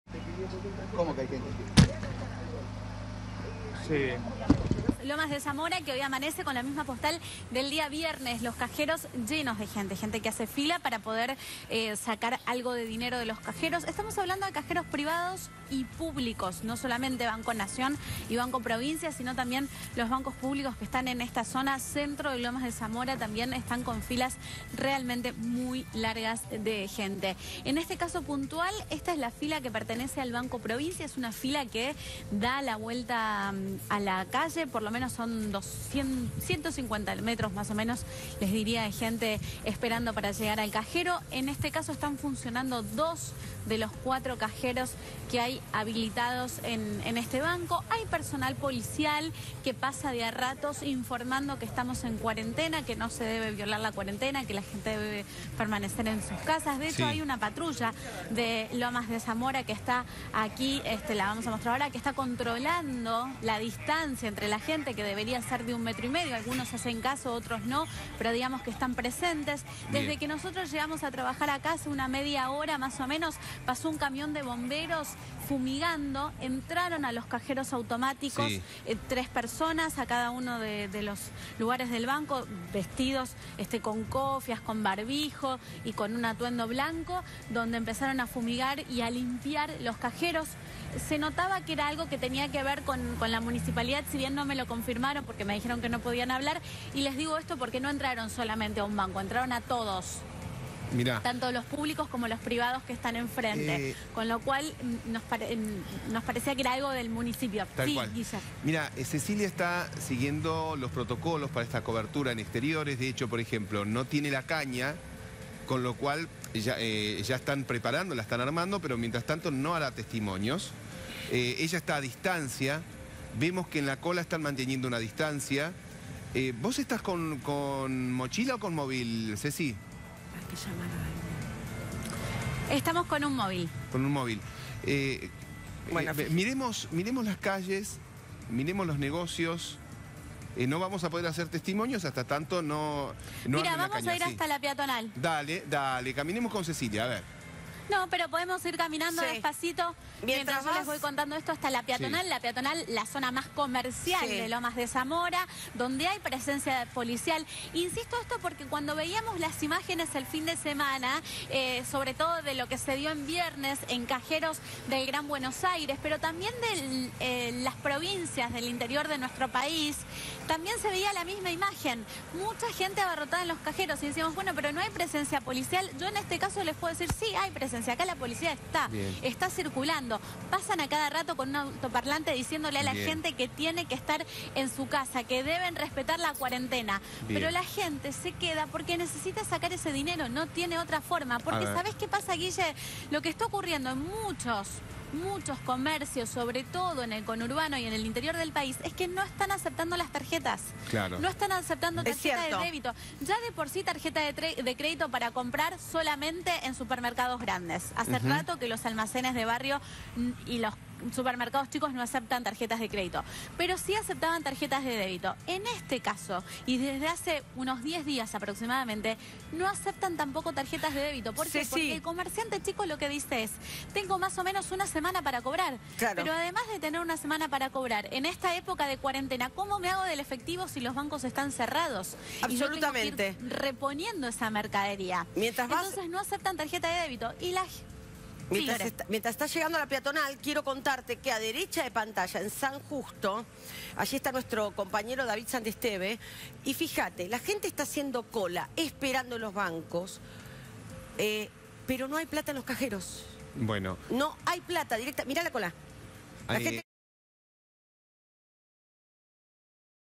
Coronavirus | La Argentina en cuarentena: colas en cajeros automáticos en el Gran Buenos Aires
Hay filas en las cajeros automáticos de Lomas de Zamora. En el primer día hábil después de que Alberto Fernández anunciara que se extiende la cuarentena, lLa gente hace colas para sacar plata de los cajeros automáticos.